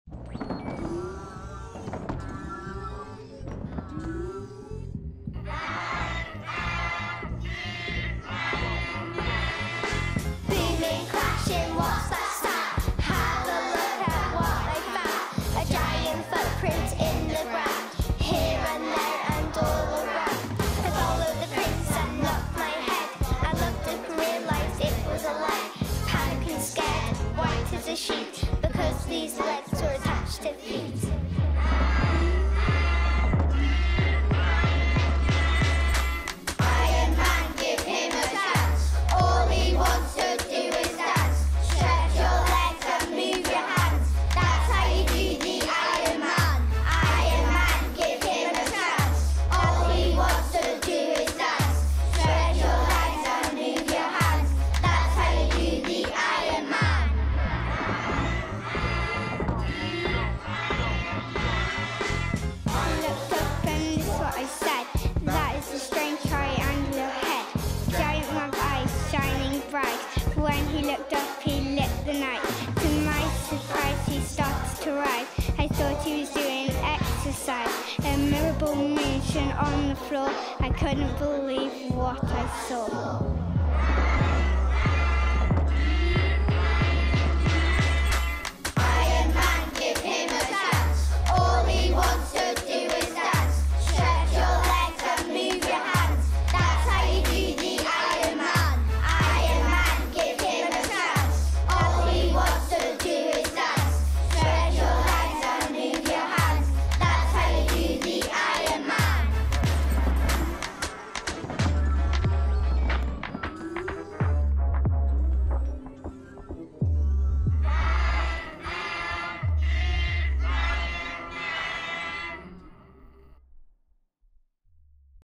Creative Schools Project Iron Man
A mixture of rap and art to think more creatively about the Iron Man story